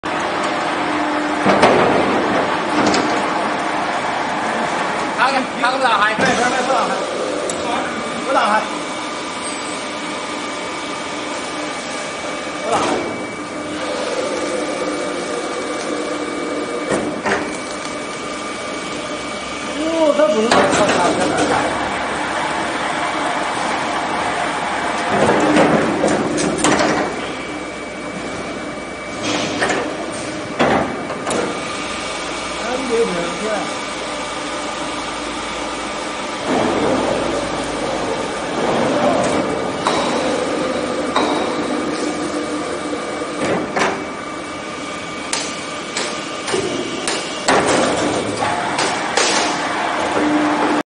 Vertical log centering charger, log centering machine
For 600-800mm diameter log, we usually use the vertical log centering machine match the single chuck rotary peeling lathe. This is an important peeling line for veneer and plywood produce.